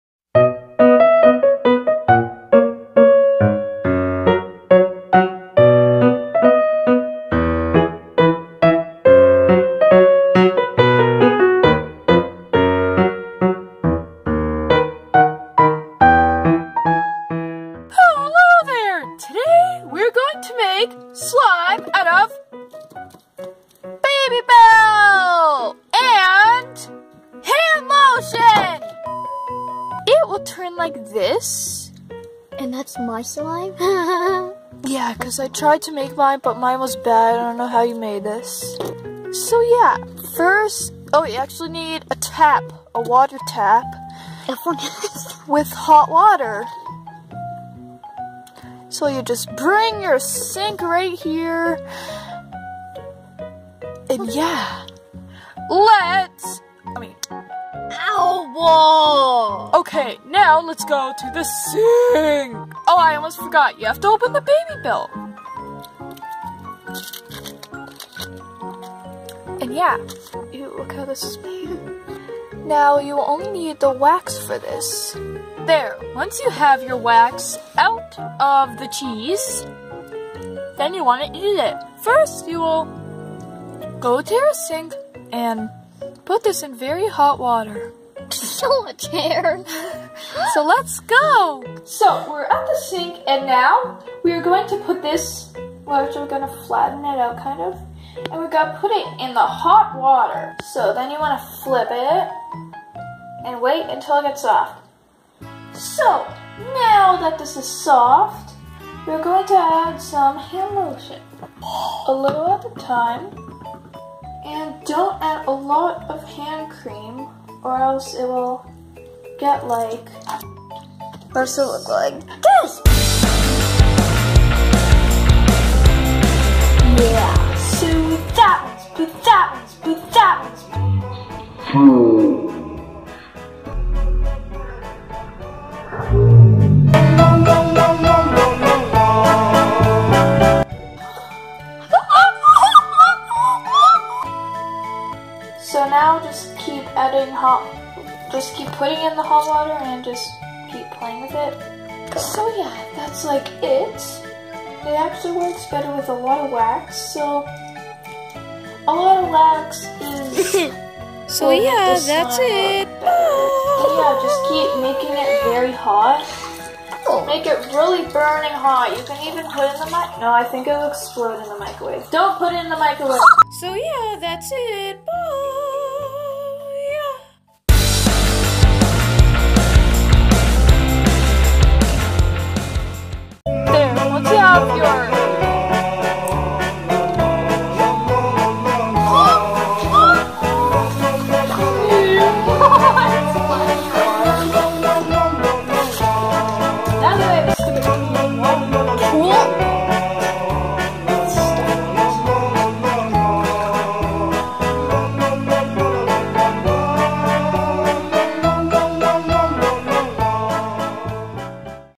How To Make (bad) Slime With Babybel (not really slime) (doesn't really work)
This doesn't even work and has so much dislikes. AND WHY DOES MY WORST VIDEO HAVE THE MOST VIEWS!

If you add water into it it will turn like my old one. SO DONT ADD WATER INTO IT

Ingredients:
- A sink for the hot water
- Hand cream or body lotion